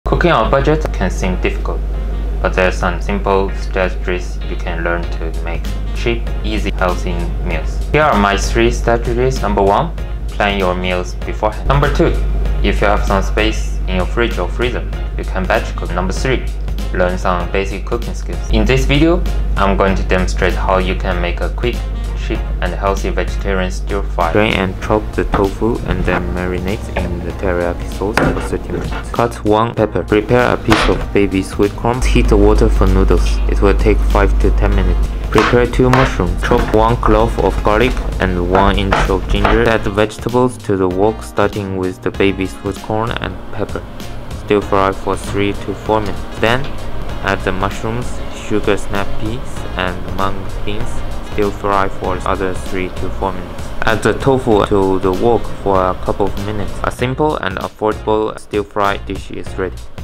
ND Cooking on a Budget Instructional Video
My presenter shows how to make a vegetable tofu stir fry on a budget in this 1 minute instructional video I made for my unit 3 exam back in April 2023.